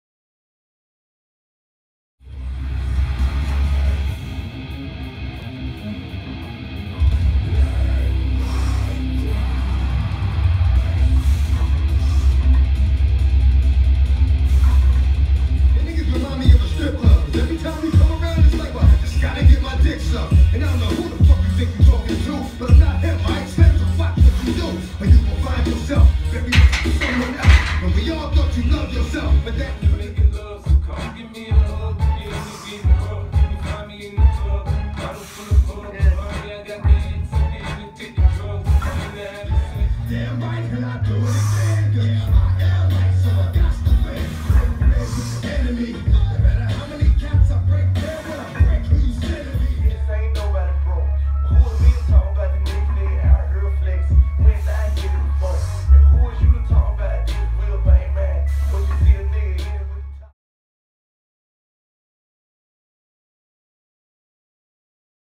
Ken Johnson shirtless SKWAAAT
Last night was heavy squat night, 325 5x2 moved good! First wave of the Cube is finished, can't wait to see where I'm at after another three weeks!